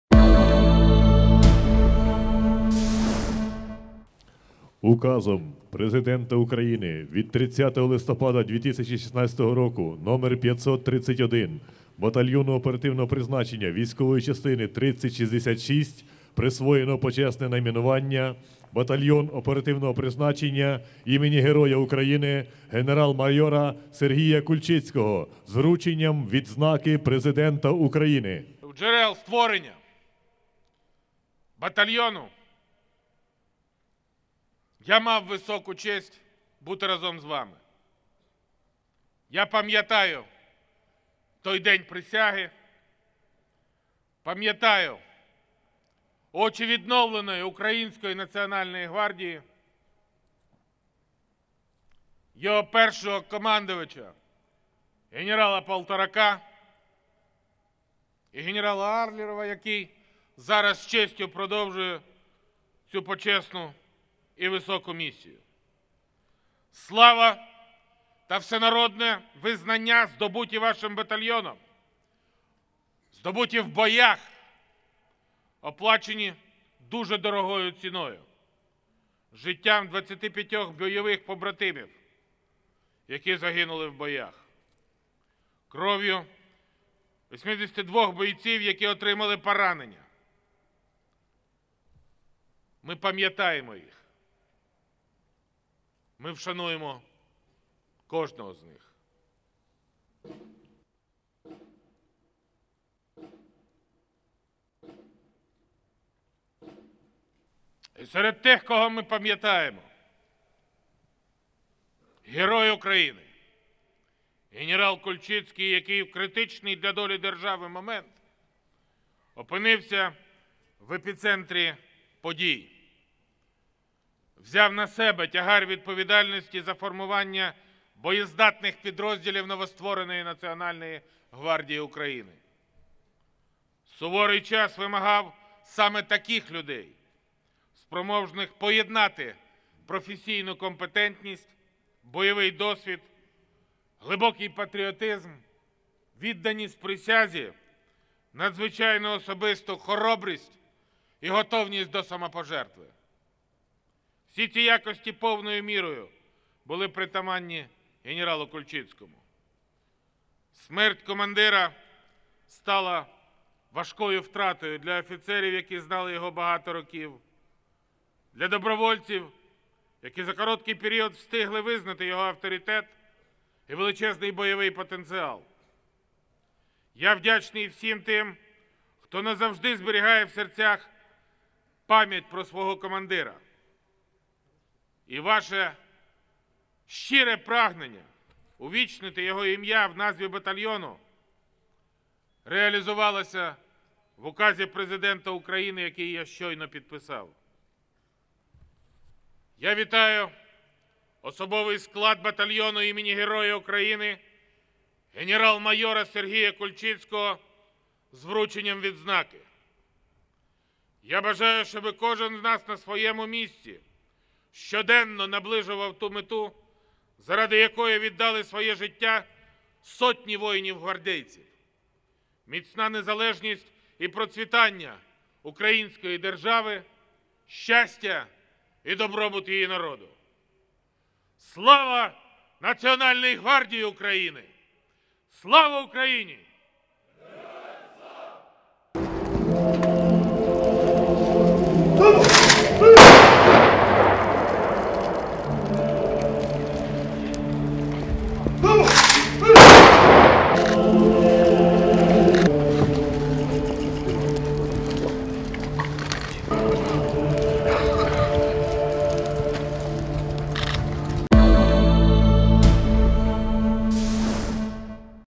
Президент України вручив відзнаку батальйону ім. Героя України С.П. Кульчицького
У Києві на території військової частини 3066, Північного ТРК Національної гвардії, відбулися заходи за участі найвищих посадових осіб країни з вручення відзнаки батальйону ім. Героя України генерал-майора С.П. Кульчицького.

У церемонії взяли участь Президент України Петро Порошенко, командувач Національної гвардії України генерал-лейтенант Юрій Аллеров, секретар Ради національної безпеки і оборони України Олександр Турчинов, міністр внутрішніх справ України Арсен Аваков, міністр оборони України генерал армії України Степан Полторак, військовослужбовці та ветерани батальйону і військової частини загалом, родичі загиблих гвардійців.